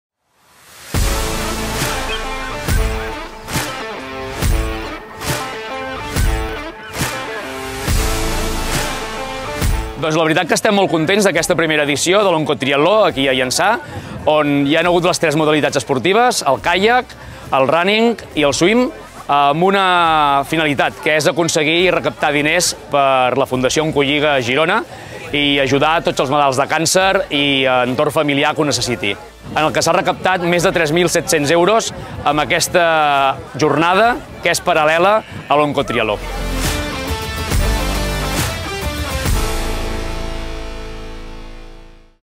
Oncotriatló 2022 (VÍDEO OFICIAL)
L'ONCOTRIATLO és un repte esportiu i solidari per equips que té com a finalitat recaptar fons per finançar els serveis que la Fundació Oncolliga Girona porta a terme per millorar les condicions de vida dels malalts de càncer i els seus familiars. Els diners es recapten a través de les aportacions solidàries dels equips, que han de fer un donatiu mínim per poder-hi participar.

Aquest vídeo és un resum de la primer edició, celebrada el 25 de setembre de 2022.

L’esdeveniment inclou tres modalitats esportives (5 km en caiac, 10 km de córrer i 2,5 km de natació en aigües obertes) i està promogut per la Fundació Oncolliga Girona amb la col·laboració de la delegació local de l’entitat a Llançà, l’associació Tintoreres Swimming Club, l’Escoleta Trail Running Cap de Creus i SK Kayak.